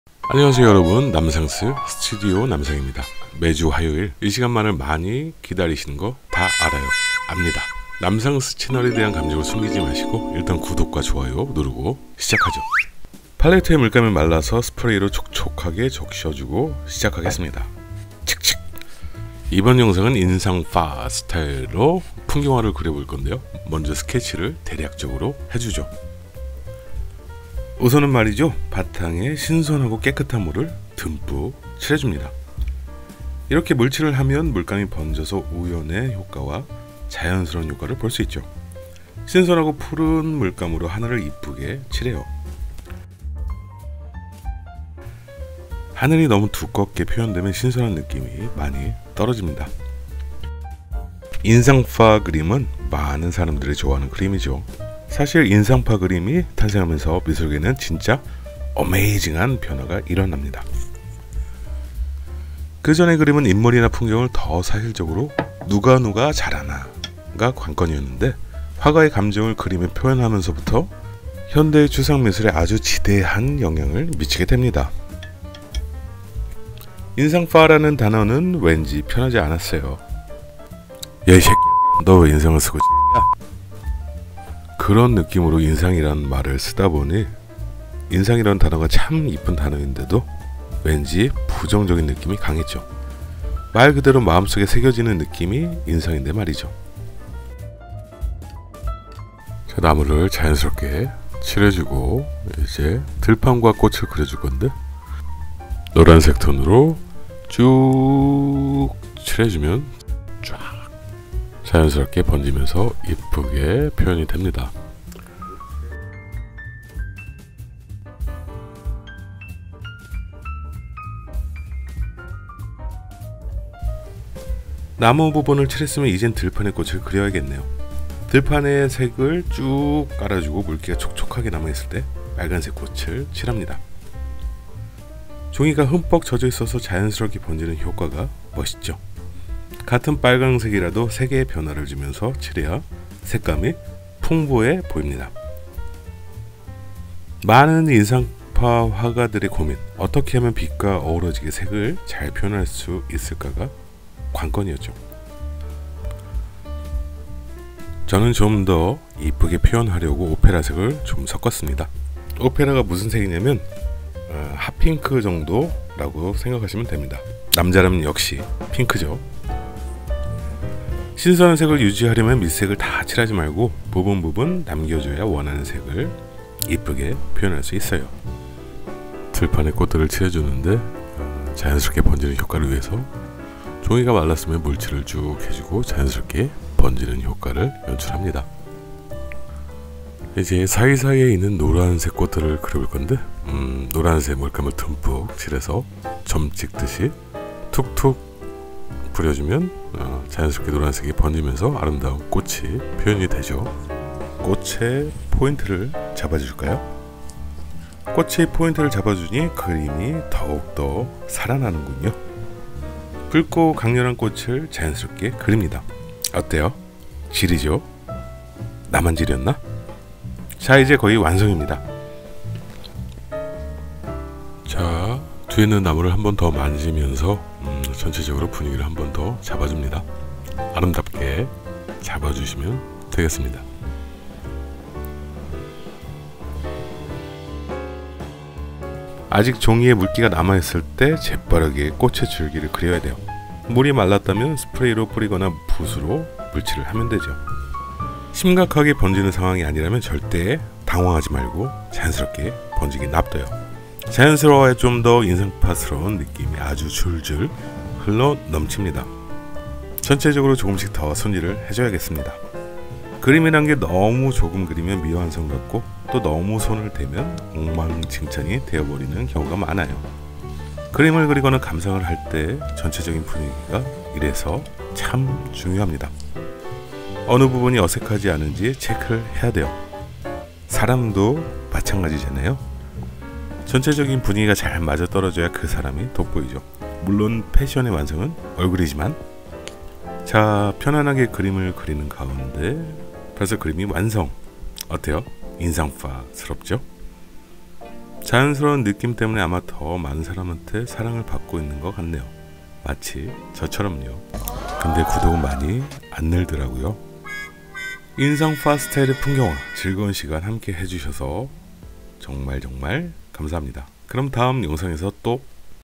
How to paint an impressionist style landscape
This video is a video showing a landscape painting in the style of impressionism.
You can all paint cool Impressionist styles like Monet and Degas!
Please enjoy watching and think of the impressionist feeling well~

How to paint  an impressionist style  in watercolor

Let's be an  Artist. Right Now!!   Let's do it now! Now is the  time.